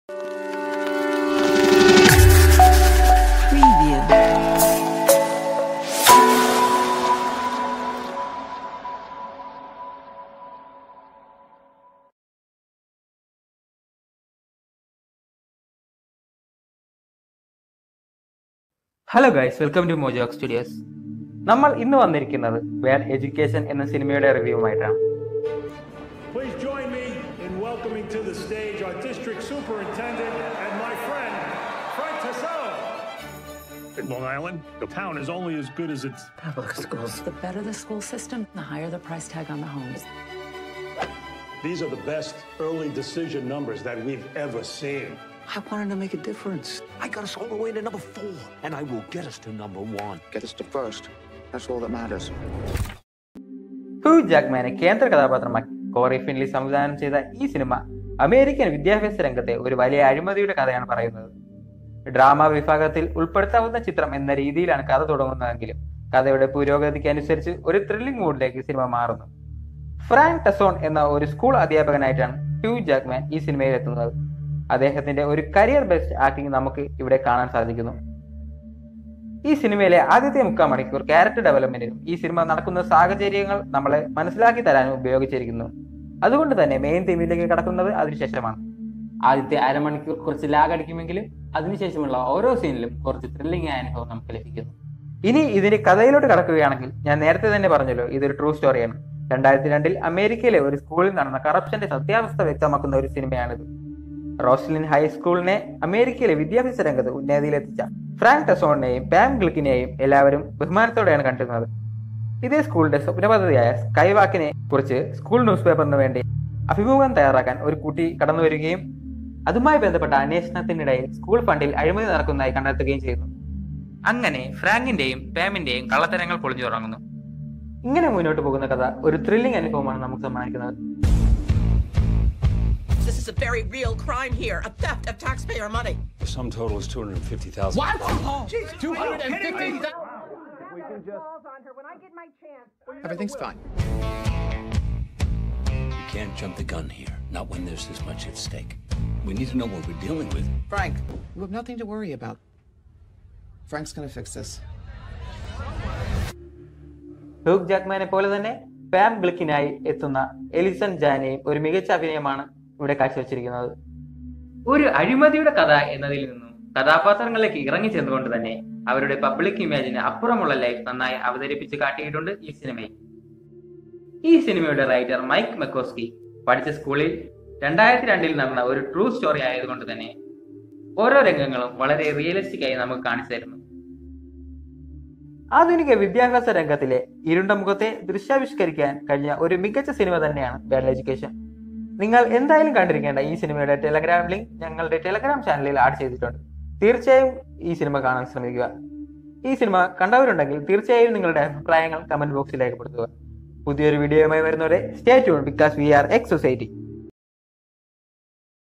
Bad Education Malayalam Review | Mojoarc Studios
Bad Education is a 2019 American crime-drama film directed by Cory Finley and written by Mike Makowsky. The film is based on the true story of the largest public school embezzlement in American history. ... The film is set in the Long Island village of Roslyn during the early 2000s.

themojoarcstudios_